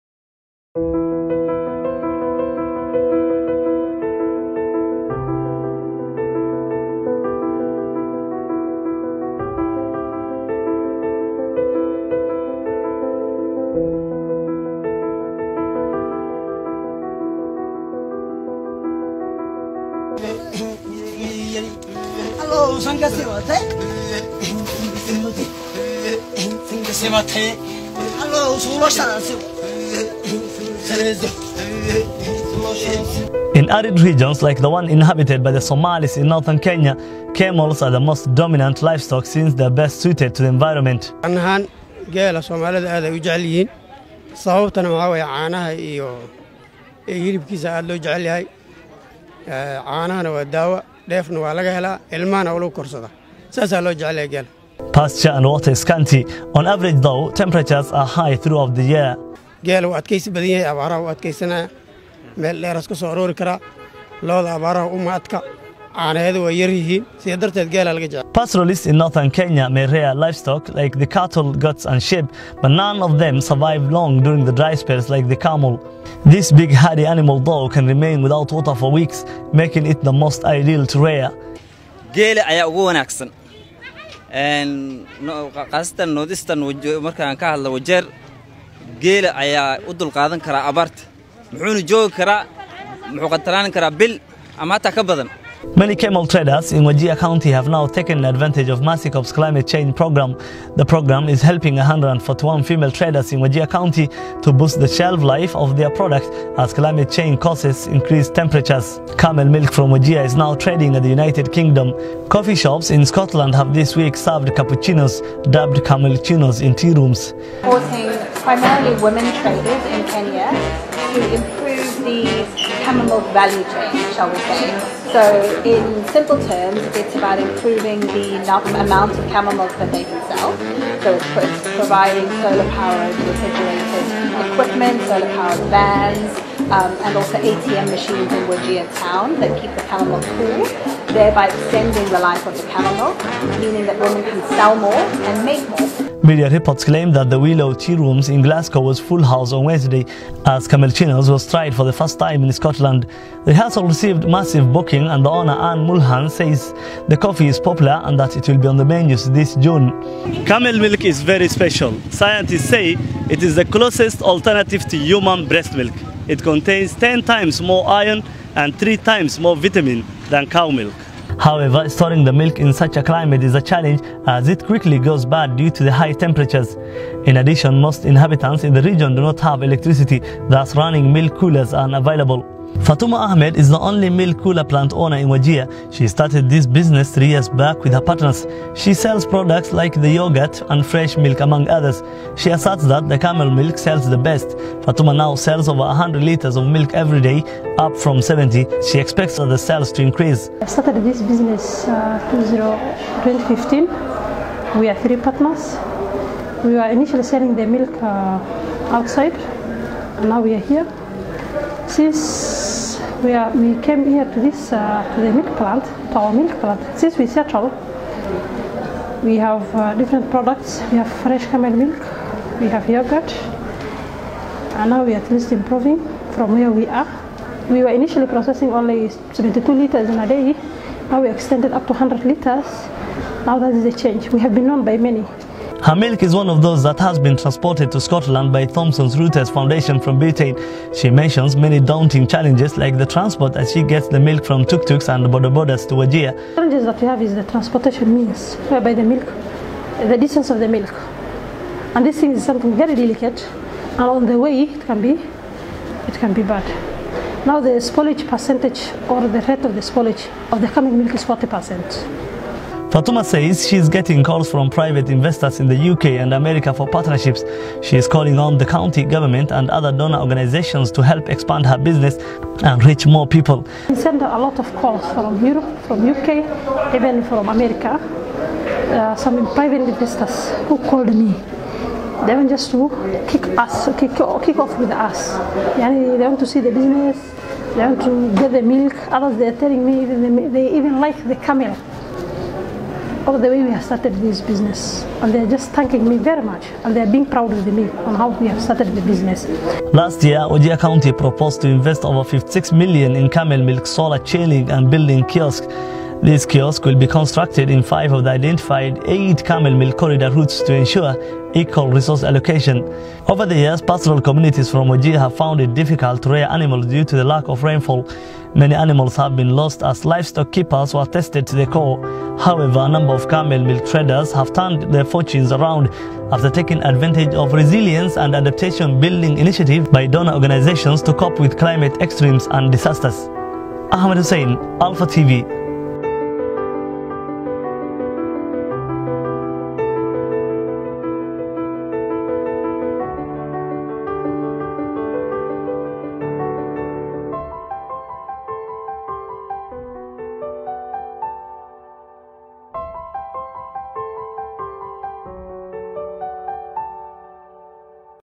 Desert To The World
Extreme Temperatures and inadequate Rainfall has Forced Wandering Nomards in Northern Kenya to Bank on Camels and their Milk- Now even a more precious commodity as the World Rushes to have a taste of its wide range of Benefits.

And as our Senior Reporter Ahmed Hussein tells us in this special story, Camel Milk from Wajir is now Taking over the hump in Europe and America.